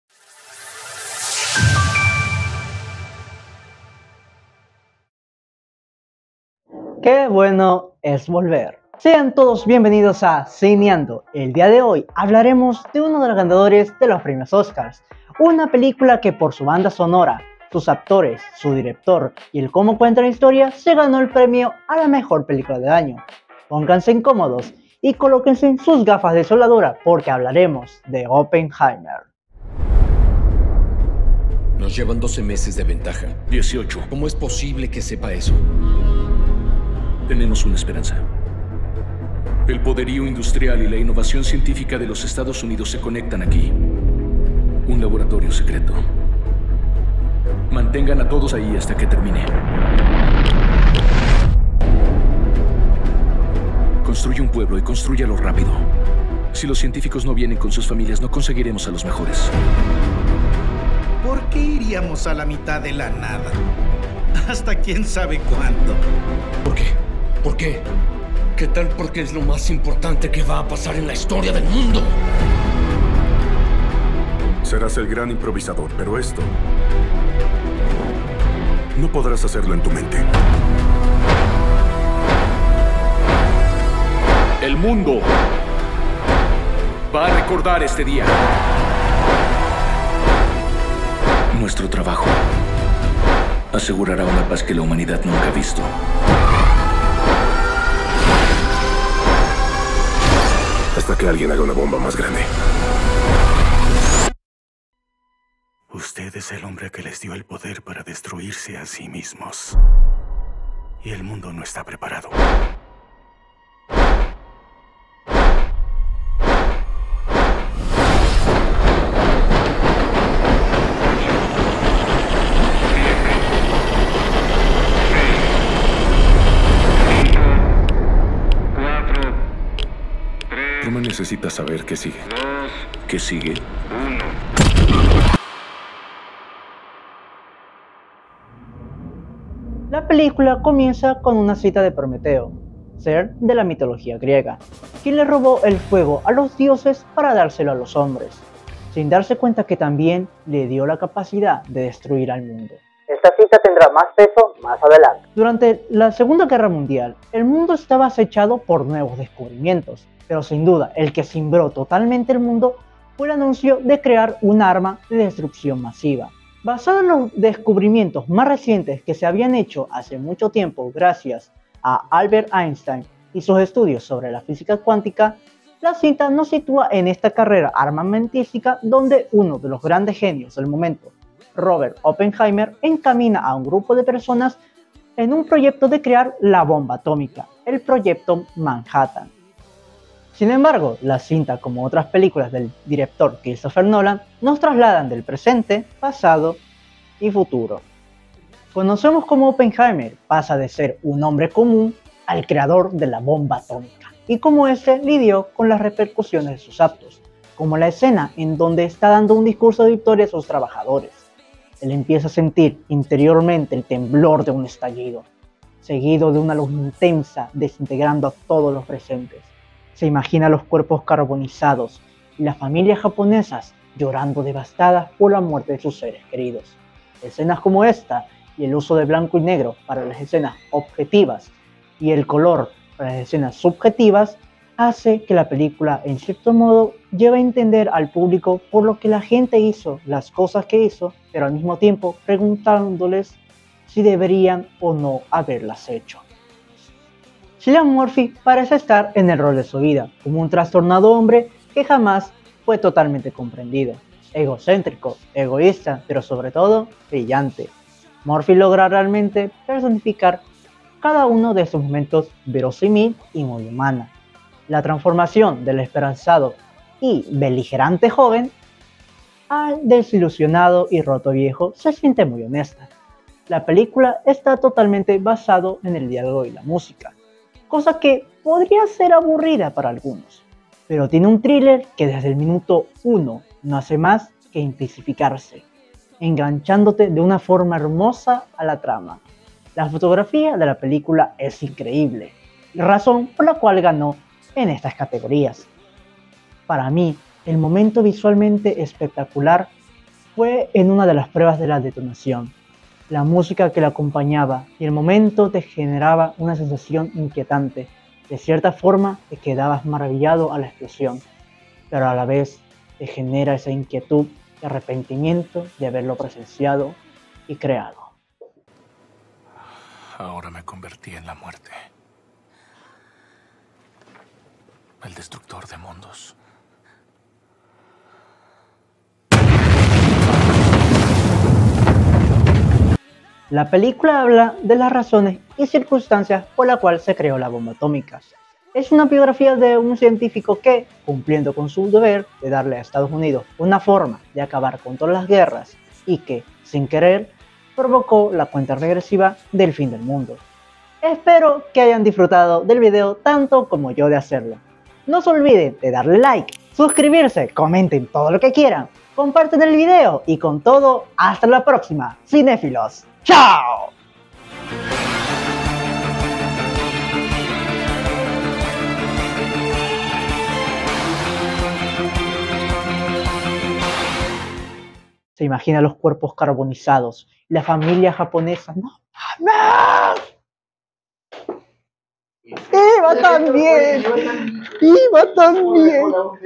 CINEANDO: OPPENHEIMER LA CREACIÓN DEL DESTRUCTOR DE MUNDOS
Sean bienvenidos cinéfilos de vuelta a cineando en su segunda temporada 🥳

El día de hoy hablaremos de uno de los ganadores de los premios Oscars 2024, una película que por su dirección🤵‍♂️, uso de colores🎨, fotografías📸 y música🎼 fue coronada como la mejor película del año, y así empezando este paseo por la alfombra roja. Con ustedes Oppenheimer

📢Redes
Instagram del canal: / istgtv_ec